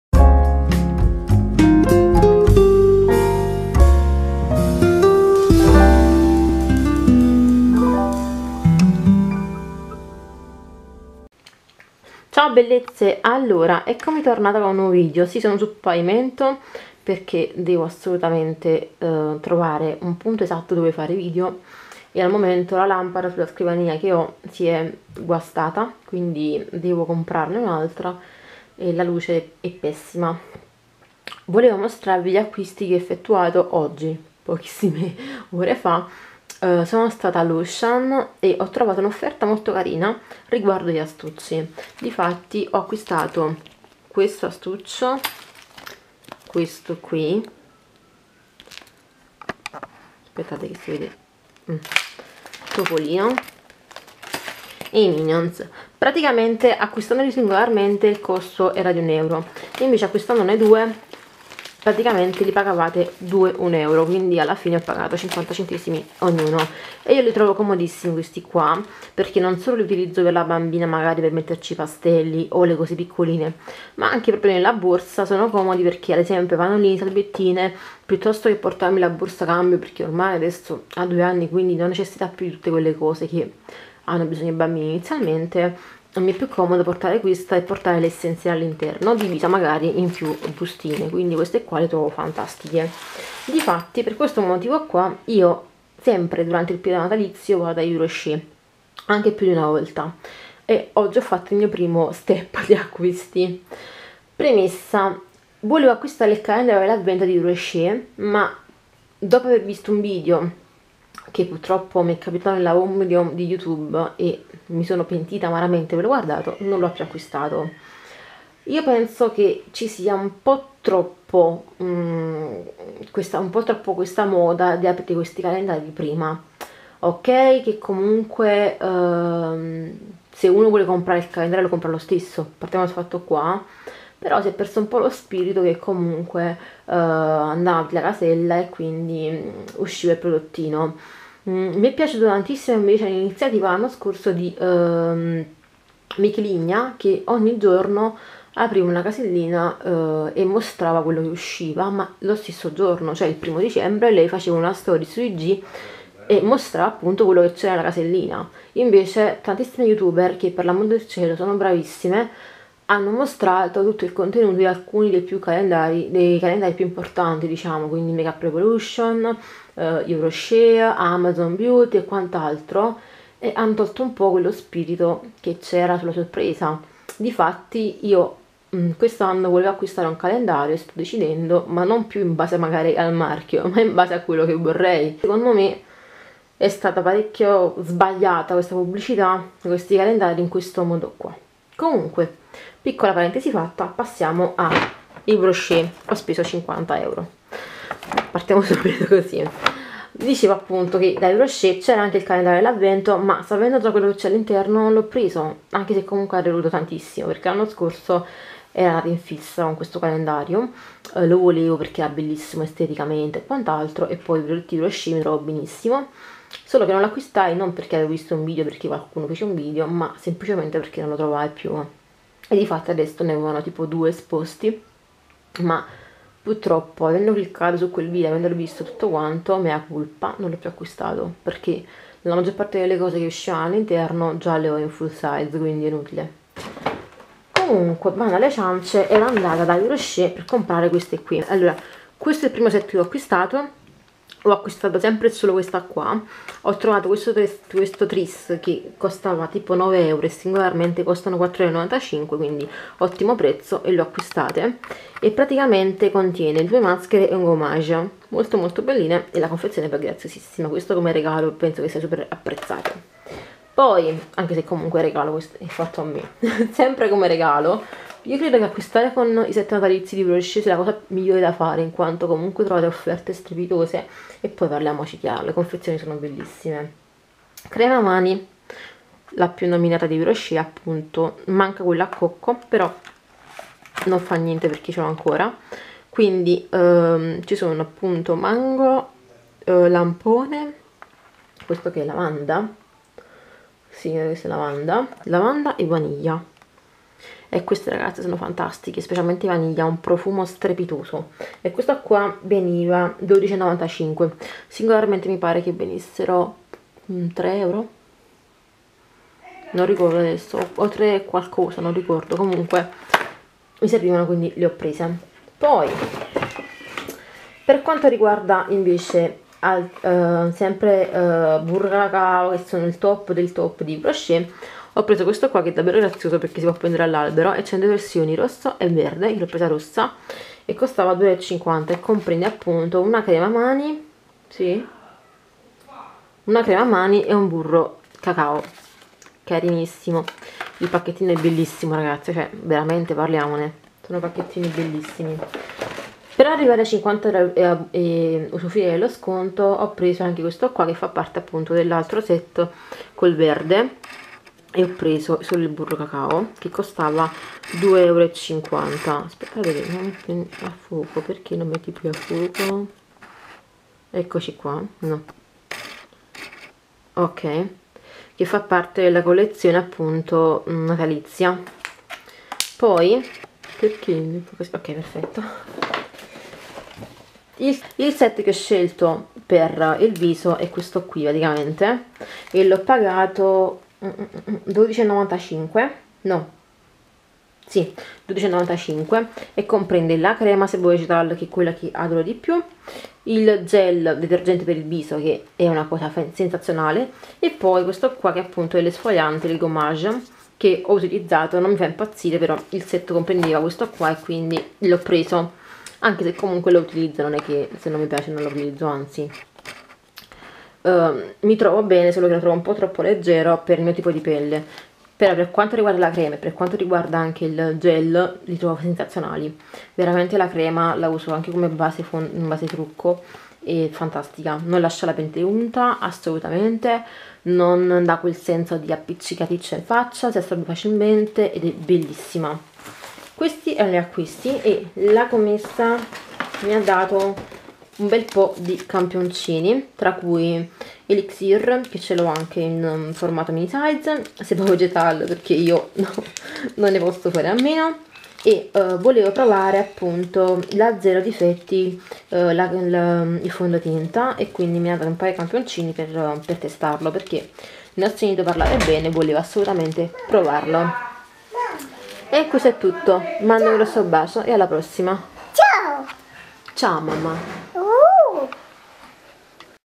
Contro i calendari dell'avvento?
INFORMAZIONI UTILI

Ciao bellezze!

Video un po chiacchiericcio in particolare riguardo i video che spopolano sul web dei calendari dell'avvento.

link diretti ai vari tutorial che ho girato sull'agenda!

istagram : laragazzaconilcodino

I PRODOTTI MOSTRATI NEL VIDEO SONO STATI INTERAMENTE ACQUISTATI DA ME.